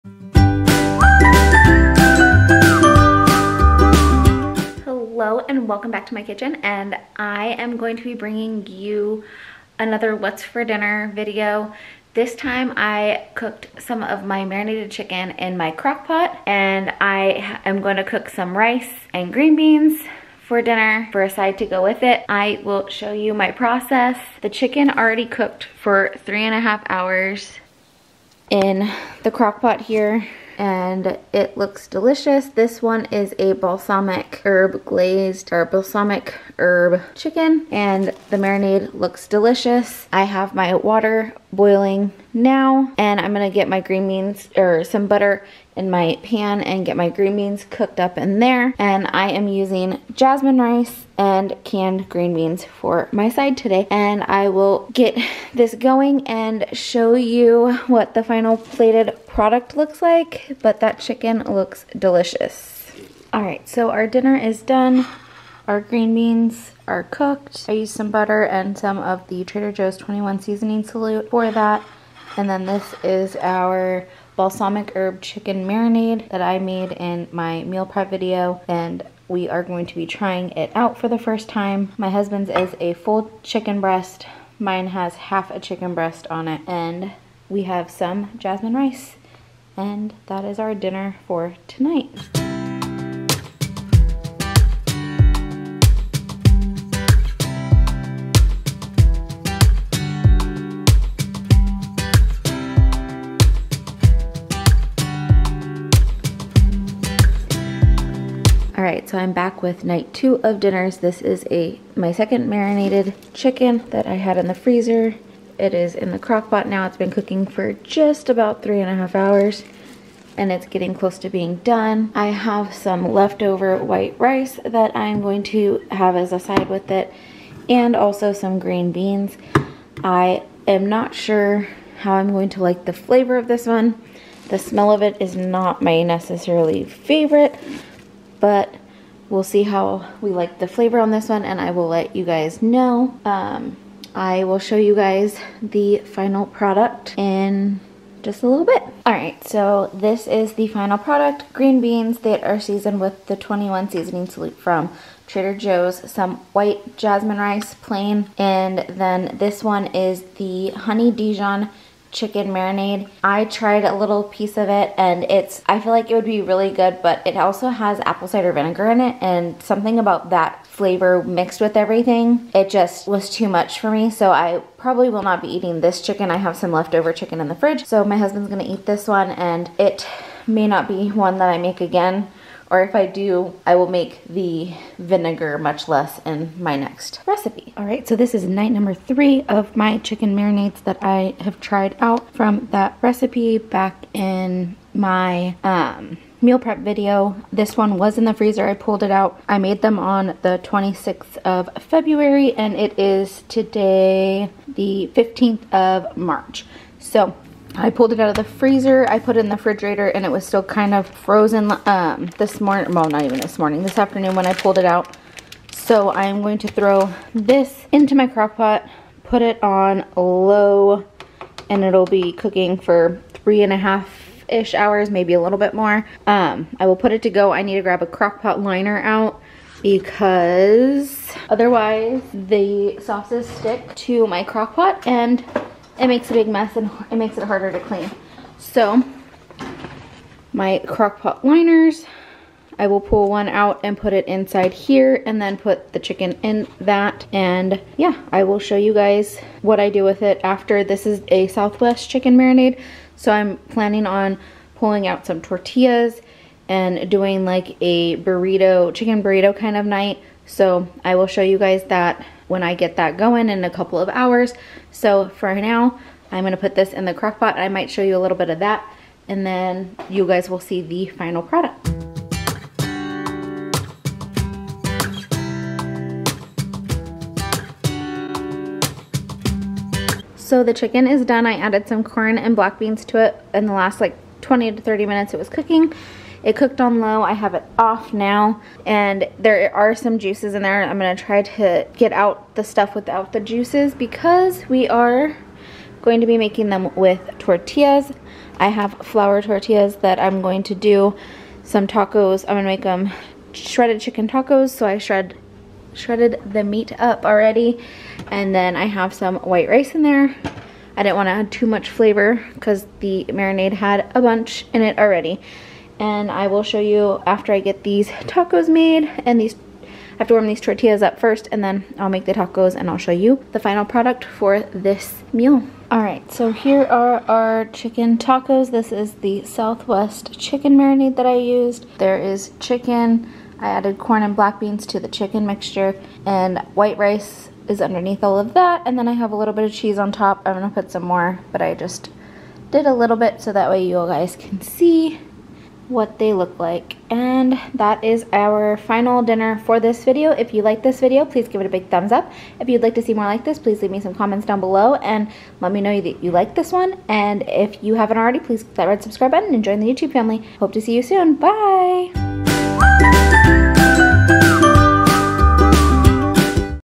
What's for dinner?- Chicken dinners- Dinner with The Weiler's - Brianna Weiler
Hi all I hope you all enjoy this what's for dinner, Leave me some comments of what you would like to see more of on these what's for dinner videos. love you all and appreciate all your support  and if your new here I love you too! ☺️ ❤️